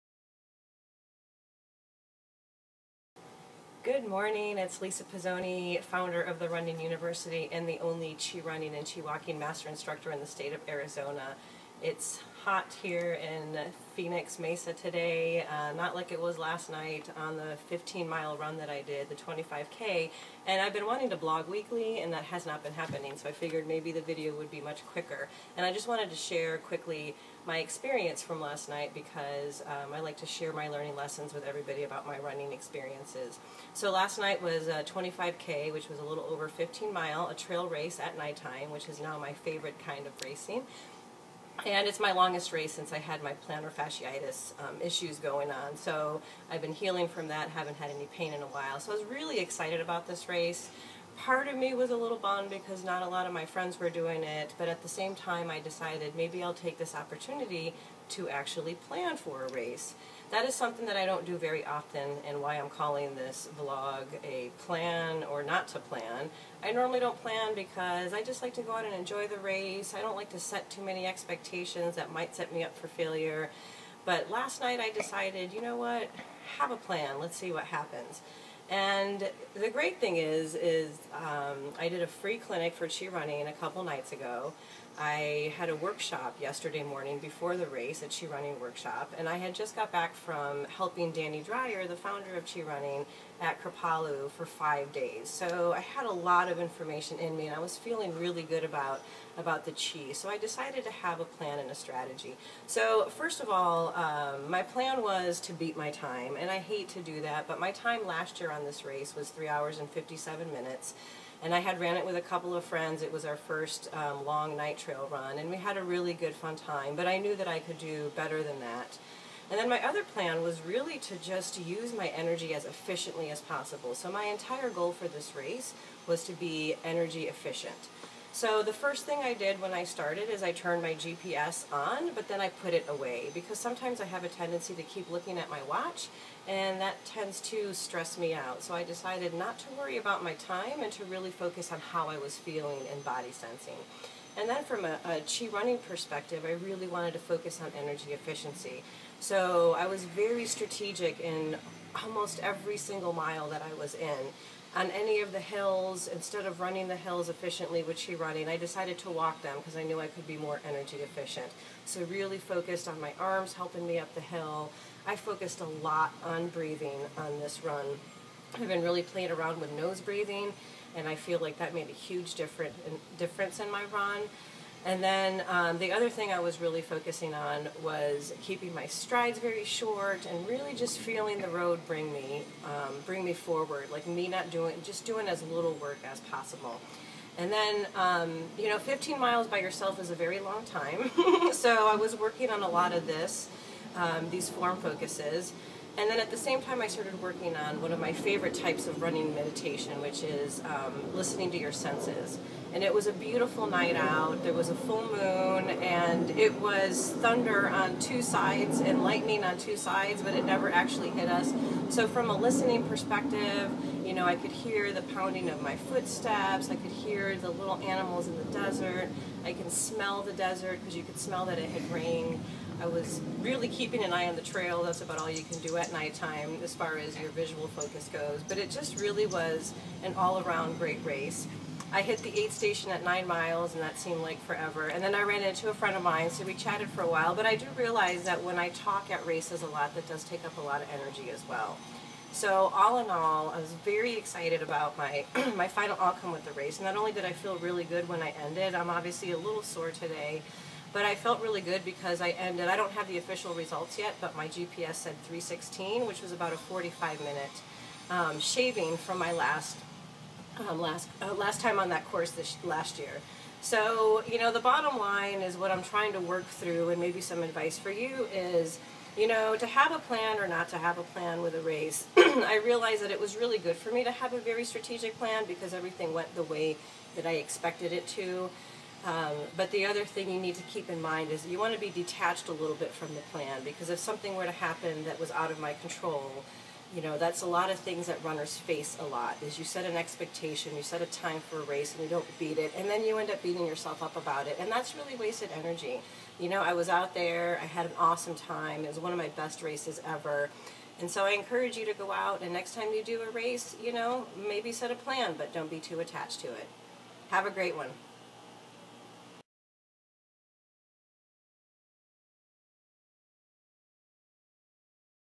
To Plan or Not To Plan? That is the Question!
Some of my Aha! Moments from Aug 2015 Aravaipa Javelina Jangover 25K night trail race:

0:31 Working around my Plantar Fasciitis
1:40 Strategy Plan: 1. Beat my previous best time
1:58 Strategy Plan: 2. Be more energy efficient
2:35 Walking the hills
2:46 Focused on my breathing techniques
2:57 Focused on my stride length and speed
3:08 Form Focuses and how they helped me
3:50 Talking expends quite a bit of energy
4:01 Results from my strategic planning
4:20 What I learned - my advice to you is try having a plan, but remain detached from any expectations of the outcome